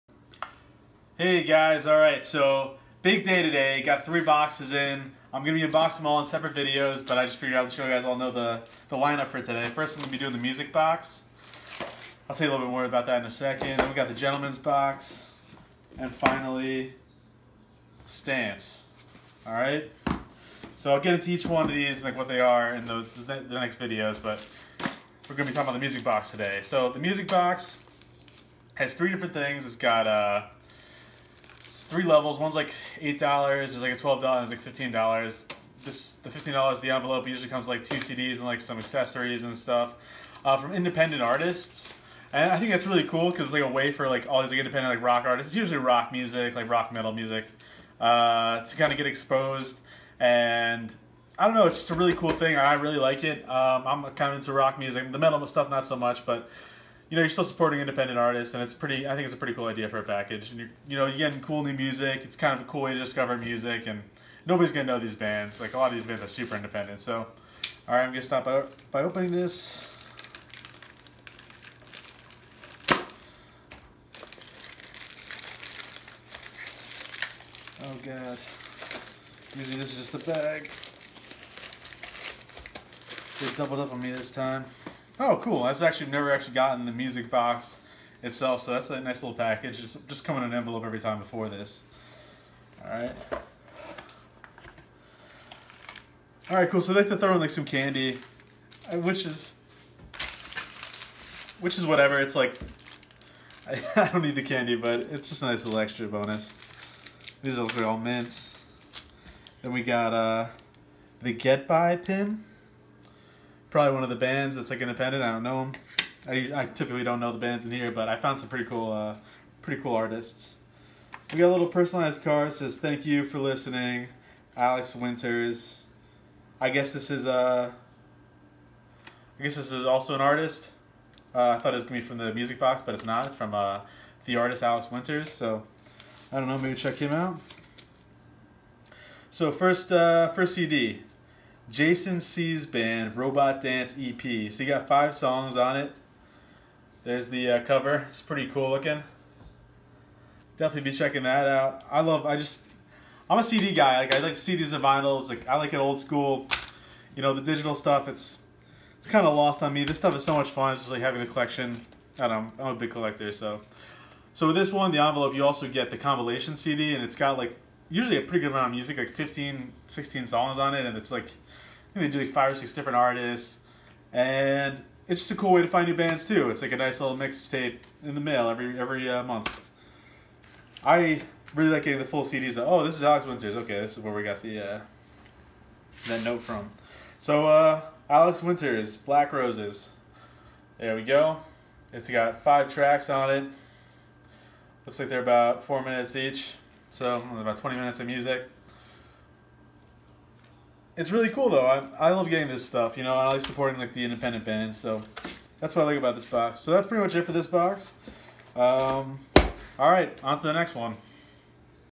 Mega Subscription Unboxing Day Part 1: The Music Box unboxing
Got 3 subscription boxes in one day Unboxing them all in a row. First up The Music Box. I screwed up in the video the tiers are:
$6 the envelope-mix cd
$10 the music box-1 artist cd and mix cd
$16 ultimate music box(what I got)-2 artist cds, mix cd, and candy/other mini things